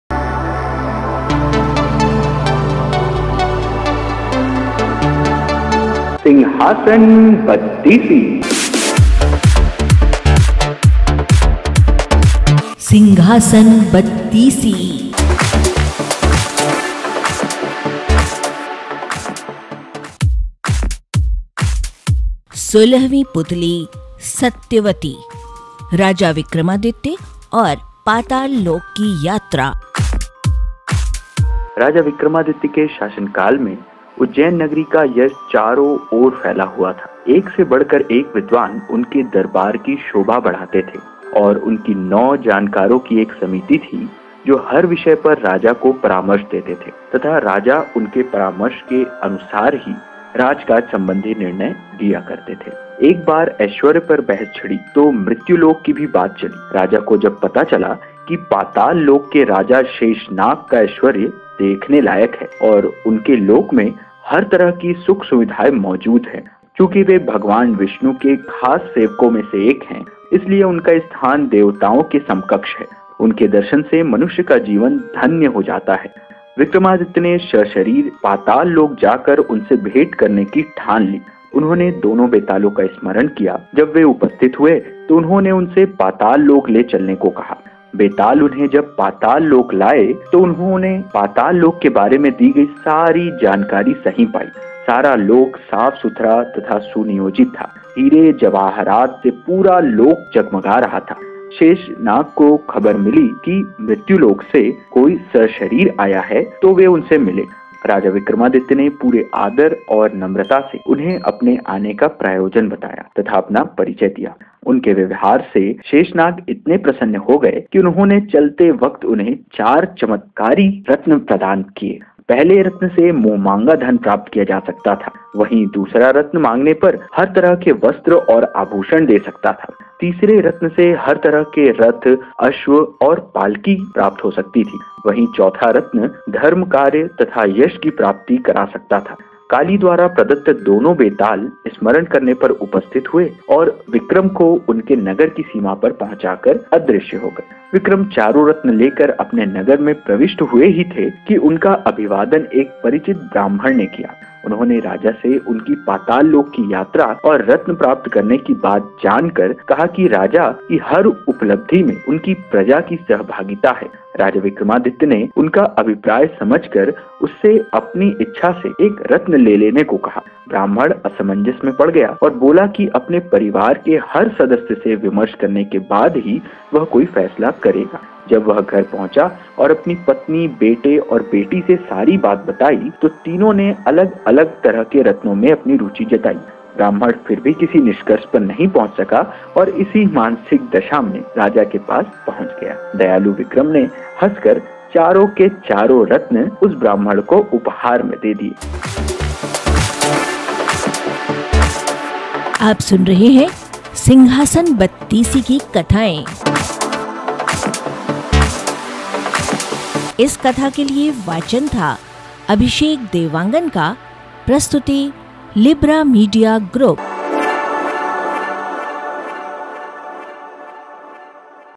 रोज़ एक कहानी : सिंहासन बत्तीसी : सोलहवीं पुतली सत्यवती की कथा
Singhasan Battisi stories in Audio
Episode  17  -Story by 16th statue Satyavati
विक्रमादित्य और पाताल लोक की यात्रा
Voice - अभिषेक देवांगन
Production - Libra Media Group